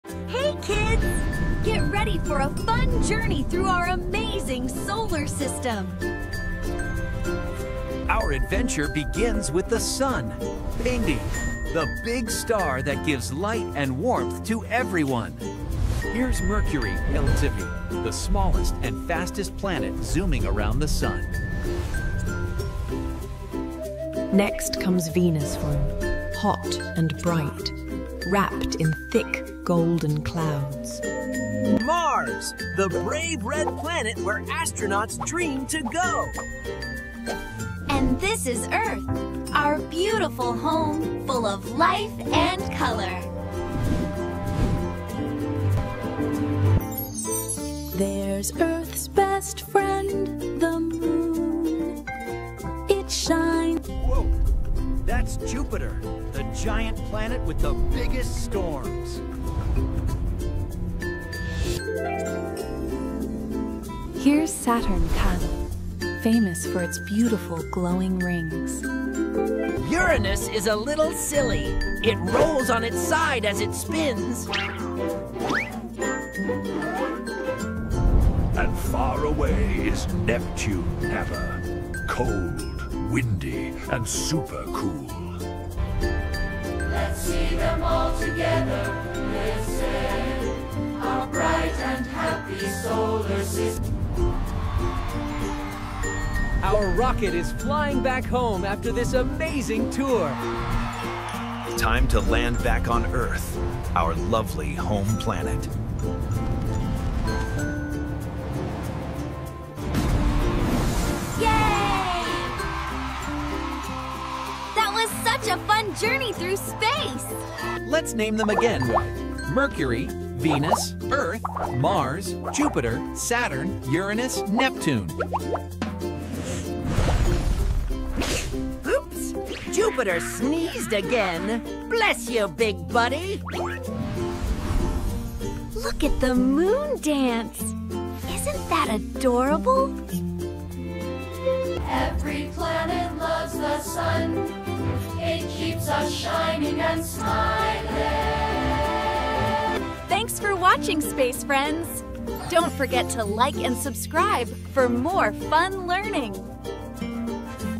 🚀 Our Solar System Adventure! | Fun Science Learning for Kids | Edu Kid Galaxy
🌞 Blast off into space with us on an exciting, colorful journey through our Solar System!
Meet every planet — from tiny Mercury to mighty Neptune — each one brought to life with big funny eyes, Pixar-style animation, and cheerful voices!

👩‍🚀 Watch as the rocket zooms, the planets dance, and the Sun smiles while teaching you amazing facts about space!
This fun-filled adventure is perfect for kids, toddlers, and preschool learners who love science and space stories.

🎨 What you’ll learn:

The names and order of all 8 planets 🌍

Why the Sun is special ☀️

Fun facts about space and movement 🚀